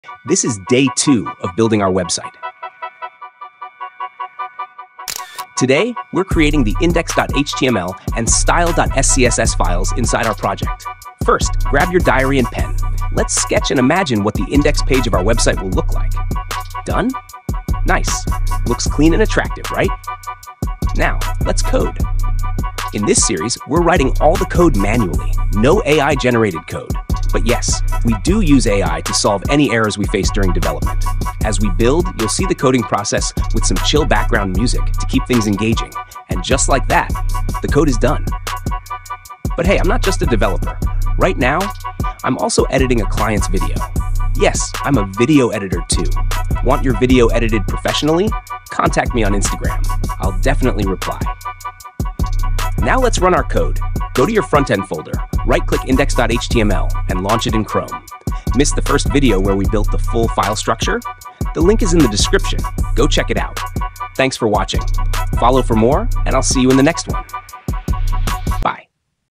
Build index.html & SCSS | Manual Coding for Website (No AI Code Used!)Day2Build
In this video, we’re designing and coding the homepage (index.html) and styling it with style.scss.
No code generators. No templates. This is manual, clean code—perfect for beginners and aspiring developers.

We start by sketching our layout with pen and paper, then write the full HTML and SCSS manually.
To keep things realistic, we’re using AI only to fix errors, not to write the code.

This website is being built for editors and content creators—and it’s coming soon!

In this video, you’ll learn:

How to design your homepage structure

How to create index.html and style.scss files

Why manual coding builds real skill

How to run your HTML in Chrome

BONUS: I show you my real client video editing work too!

My Services:
Yes, I’m a video editor too. Want your video edited professionally?

Watch Day 1 (File Structure):
[Link to Day 1 video]

Timestamps:
makefile
Copy code
0:00 - Welcome to Day 2
0:08 - Sketching the index layout
0:20 - Creating index.html
0:35 - Styling with SCSS
0:50 - Testing in Chrome
0:55 - Video editing client project
1:00 - Outro & Instagram contact
YouTube Tags (High-Search Keywords):
css
Copy code
Suggested Thumbnail